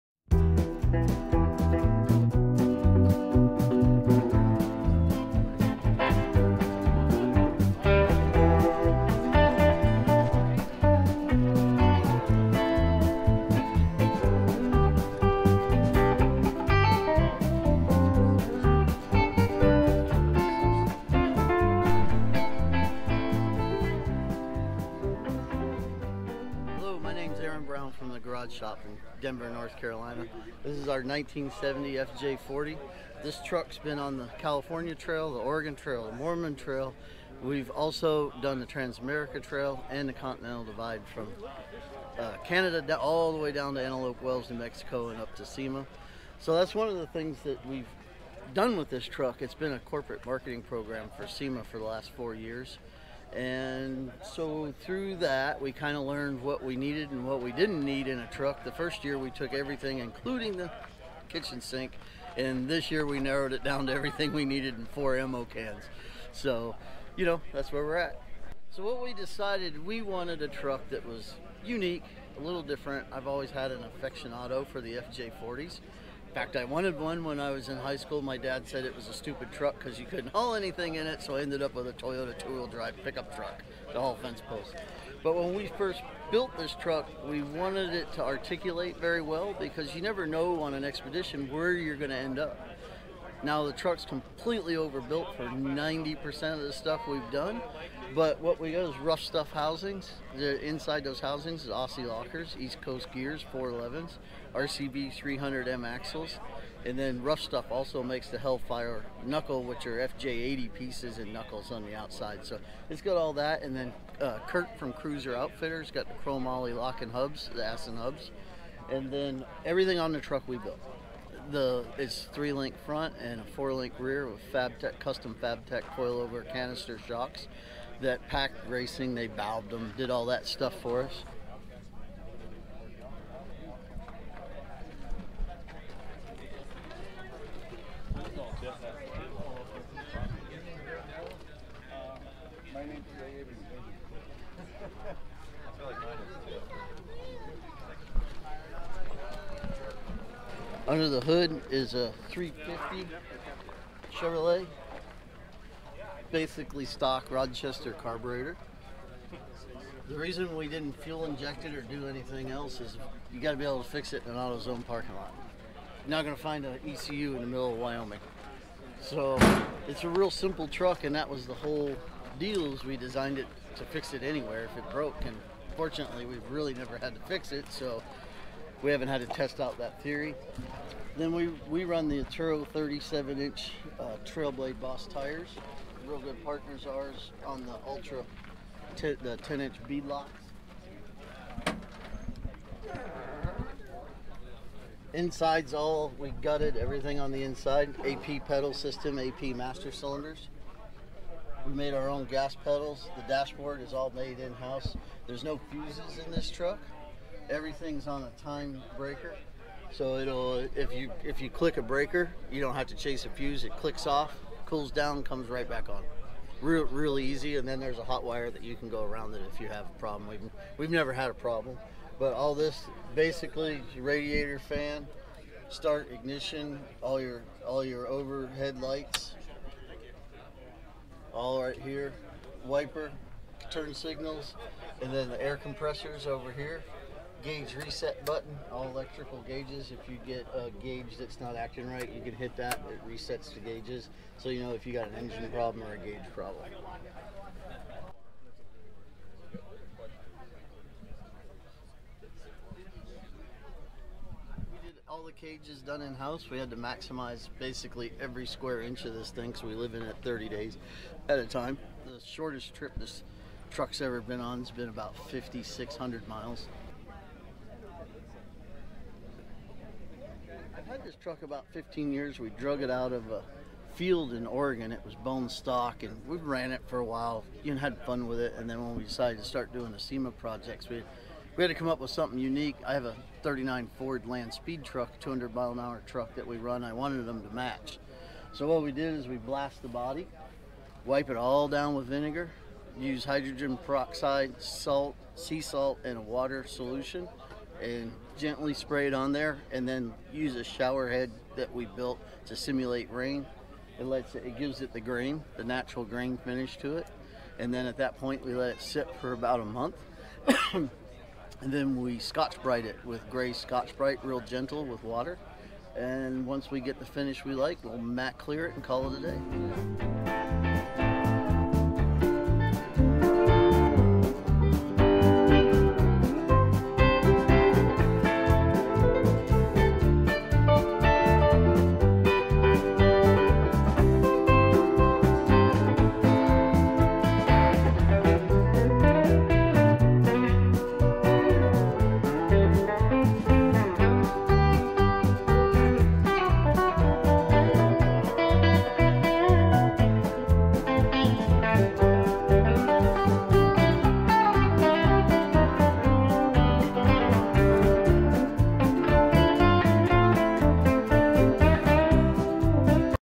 1970 Toyota Land Cruiser FJ40 w/ 350 V8 - SEMA Expedition
The SEMA Expedition Toyota Land Cruiser FJ40 was custom built by the Garage Shop of North Carolina.  It's outfitted with a Chevy 350 V8 powerplant, and custom built inside and out, including the sweet patina finish.  Aaron Brown takes us on a walkaround of this overland adventure tested fj40 that was present at Overland Expo East 2019.